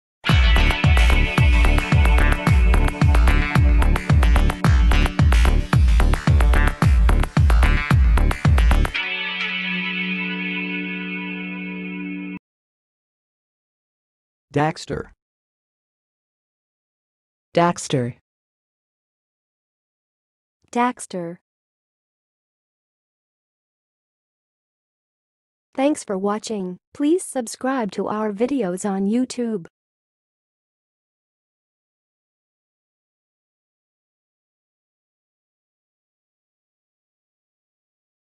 How to say "daxter"! (High Quality Voices)
Watch in this video how to say and pronounce "daxter"!